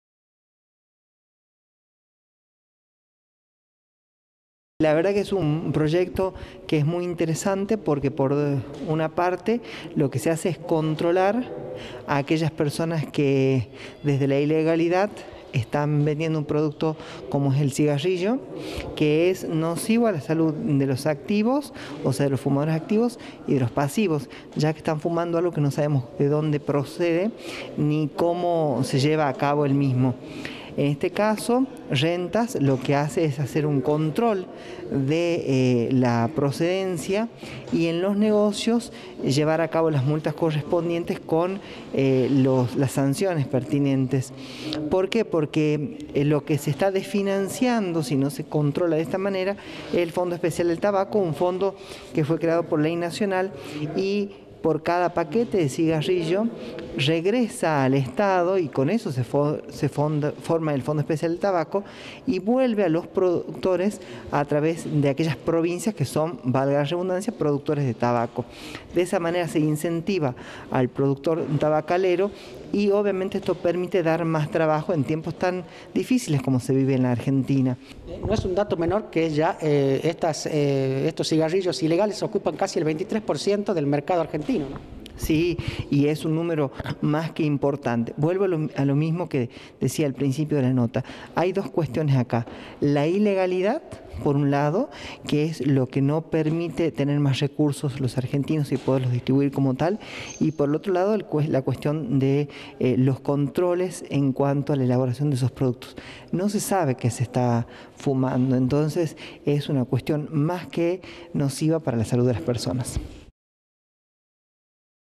CARLOS ROSSO - SENADOR PROVINCIAL
APRUEBAN QUE LA DGR PUEDA FISCALIZAR LA COMERCIALIZACIÓN DE CIGARRILLOS.